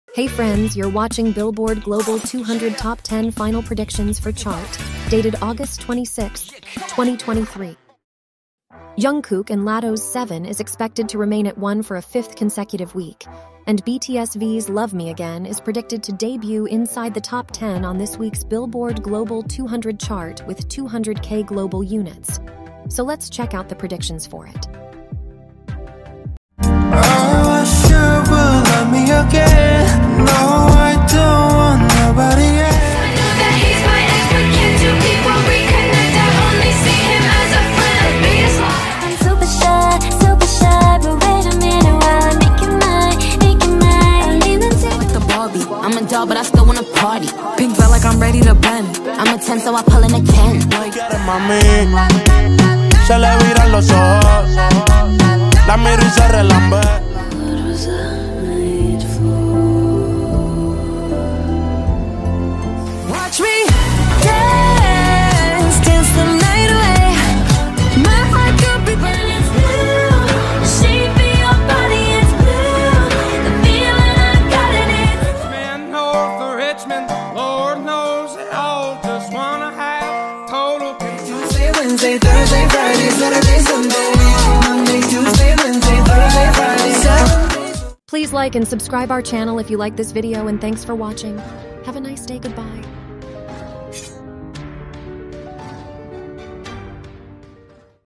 TOP 10 Billboard Global 200, FINAL PREDICTIONS | August 26,2023
These are Top 10 Billboard Global 200 single Final Predictions for chart dated August 19,2023.
It is always our endeavor to reach as much accurate information as possible related to Music World to all of you.